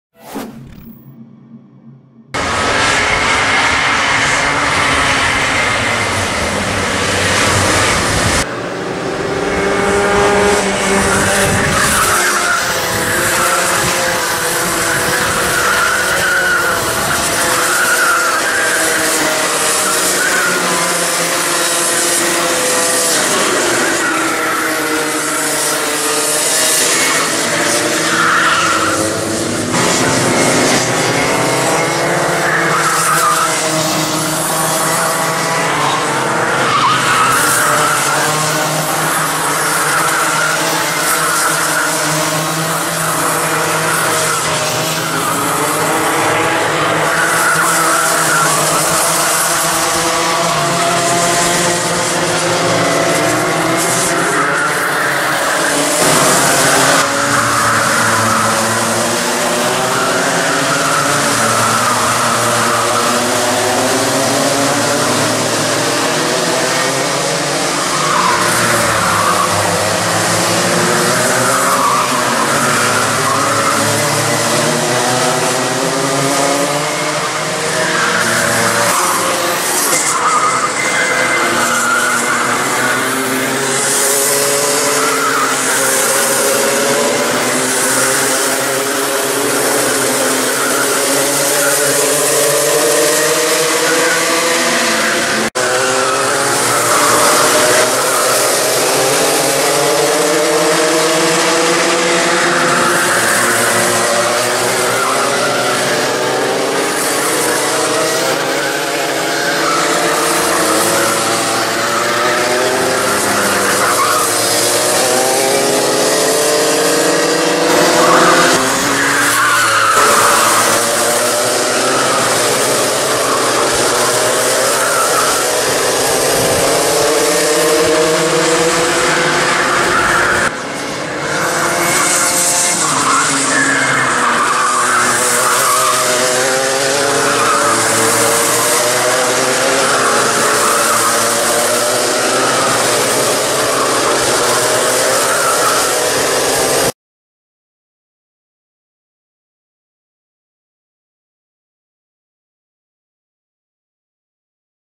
TheMotors - Course du championnat de Karting à Pers
Course du championnat Régional - Auvergne\Limousin. Dernière course qui se déroulait à Pers. Filmé par un caméscope. Bon visionnage !